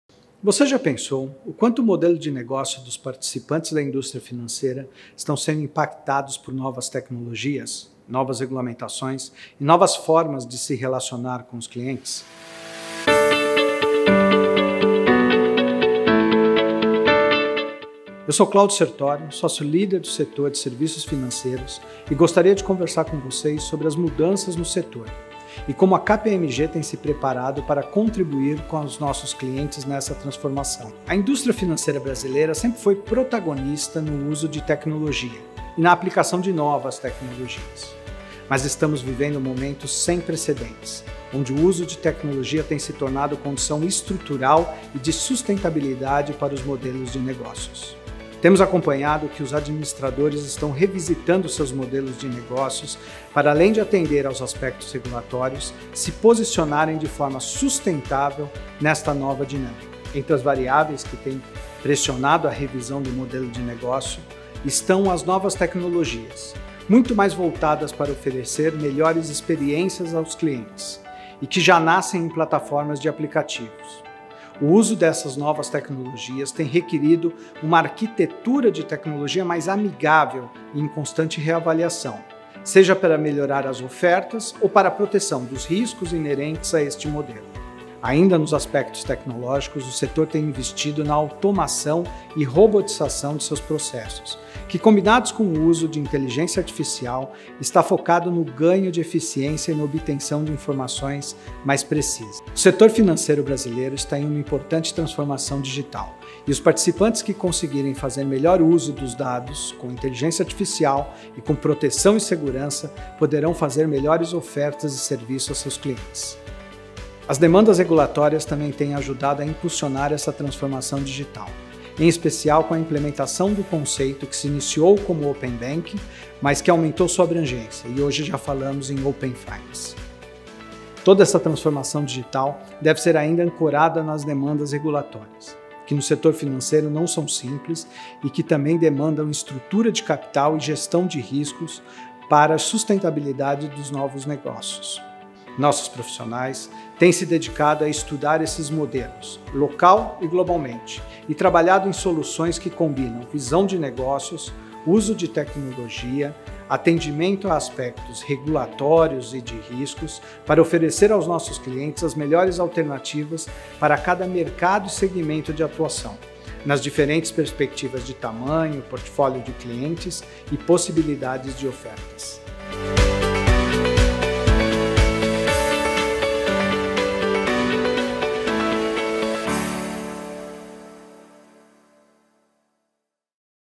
Financial Services | KPMG no Brasil
A indústria financeira brasileira sempre foi protagonista no uso e na aplicação de tecnologia e agora está sendo ainda mais demandada para o uso de tecnologia e para oferecer as melhores experiências a seus clientes. Neste contexto, é necessário passar por mudanças estruturais e revisitar os modelos de negócios, olhando para as oportunidades e riscos, para atender os clientes de forma mais dinâmica e sustentável.

Assista ao vídeo e saiba como o time de especialistas da KPMG no Brasil está preparado para apoiar seus clientes na transformação dos negócios.